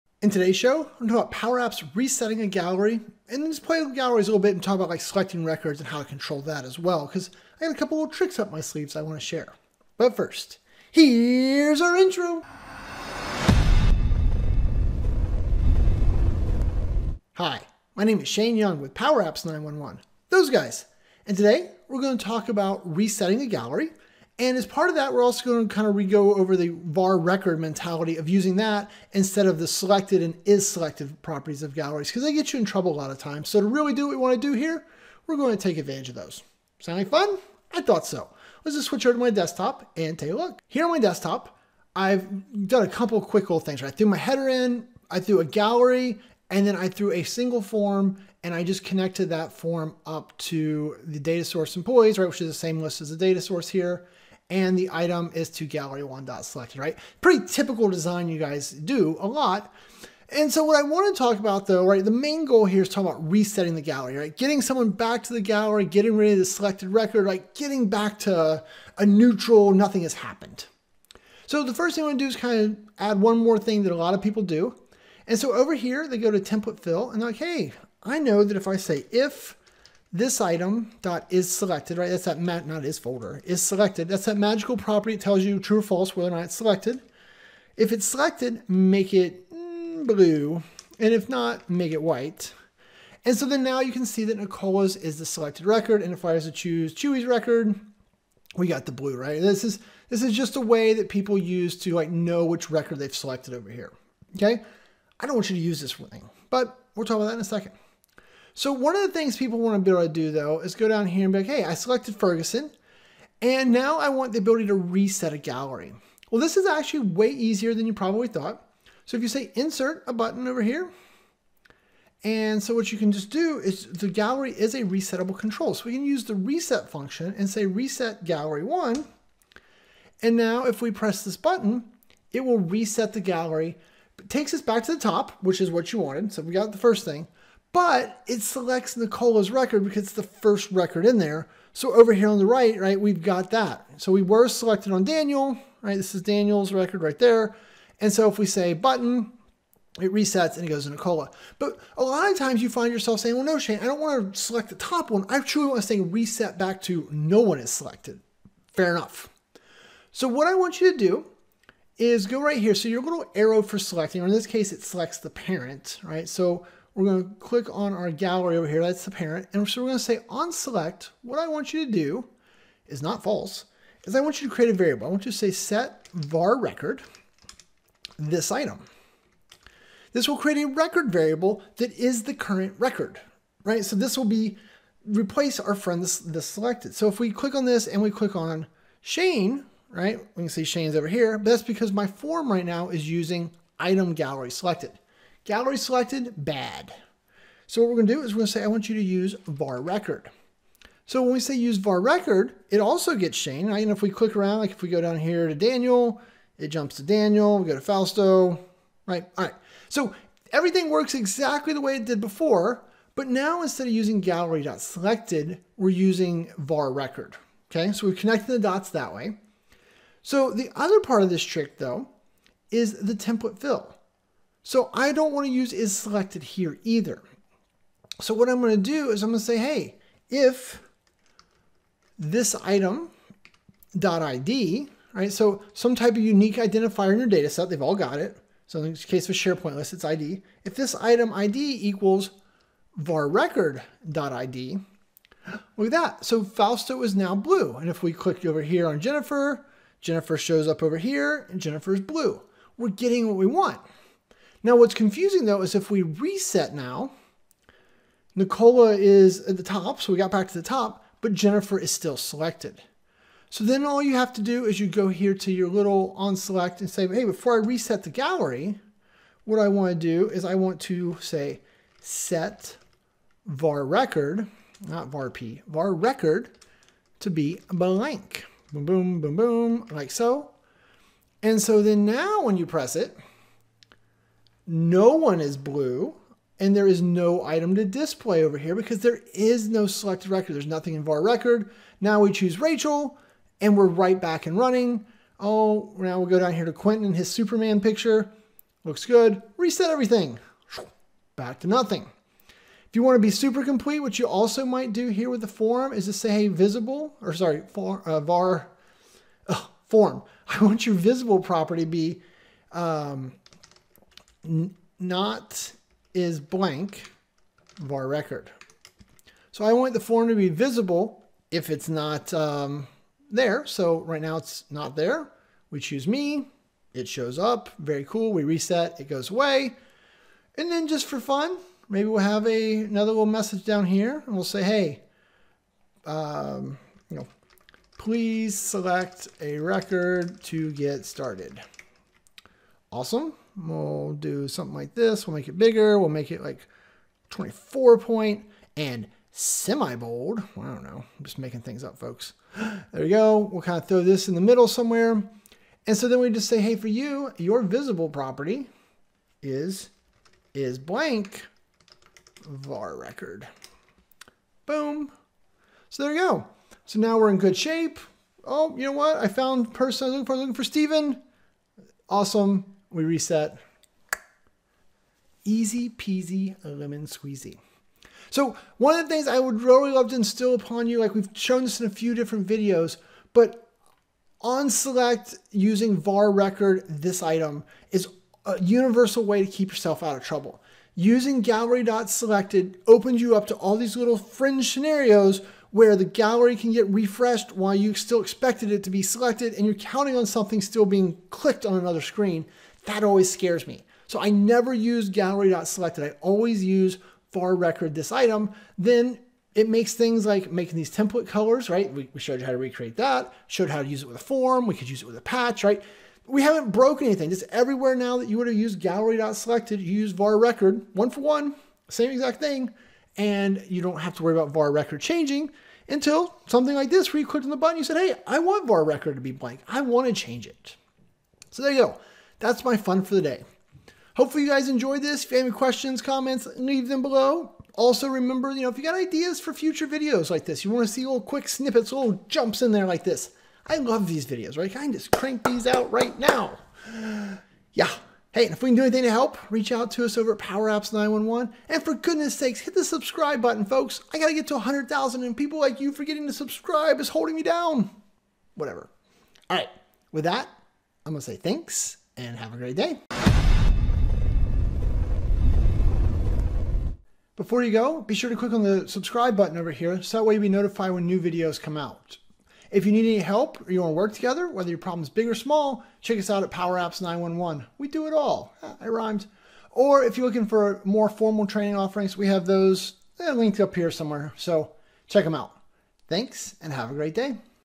Power Apps Reset a Gallery and varRecord instead of Selected
Do you ever need to "reset" your gallery but you then get confused by the selected item or how that affects the data in forms and other related controls? I bet you do. Good news I explain how to make it all better using varRecord and a couple of little changes to the way you normally build things. Pure gold here. 🥇